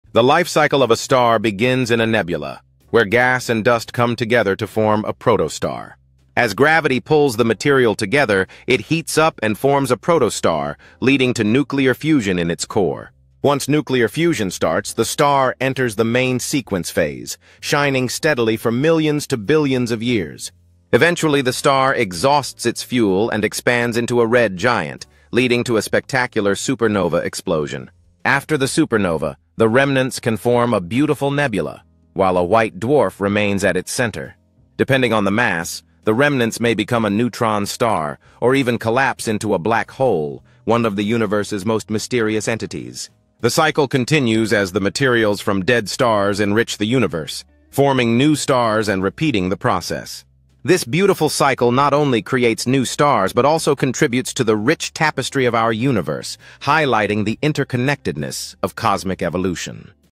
From Dust to Black Hole: A Star's Journey 🤯space
Get ready for a cosmic journey! We're exploring the mind-blowing life cycle of a star, from its birth in a nebula to its dramatic end as a supernova, neutron star, or black hole.